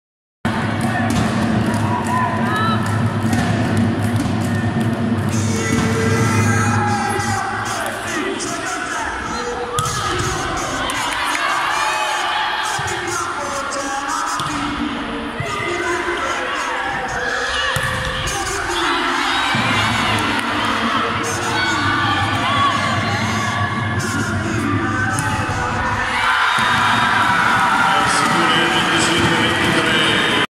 Perugia-Filottrano (fine quarto set)
ultimo punto del quarto set tra Bartoccini Fortinfissi Perugia e Lardini Filottrano di serie A1 volley femminile stagione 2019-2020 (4ª giornata di campionato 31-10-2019)